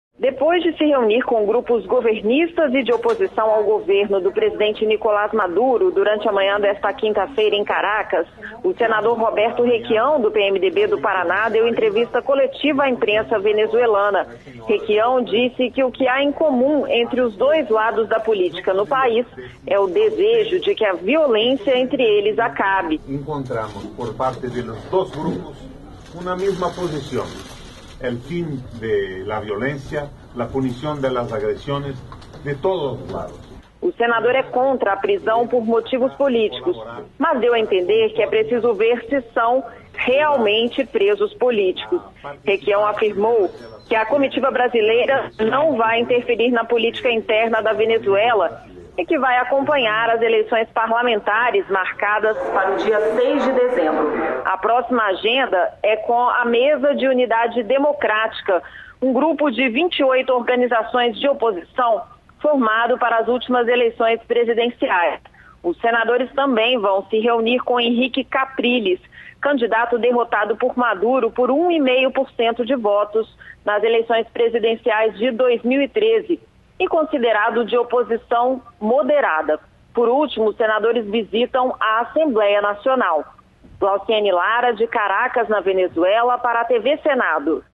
Requião: governo e oposição da Venezuela querem o fim da violência
Em entrevista à imprensa venezuelana, o senador Roberto Requião (PMDB–PR) disse que tanto a oposição, quanto o governo de Nicolás Maduro desejam o fim da violência. Requião faz parte da comitiva do Senado que visita a Venezuela. Ainda nesta quinta-feira (25), os senadores se reúnem com o candidato derrotado nas últimas eleições presidenciais, Henrique Caprilles, e visitam a Assembleia Nacional. A enviada especial, Glauciene Lara, tem mais informações.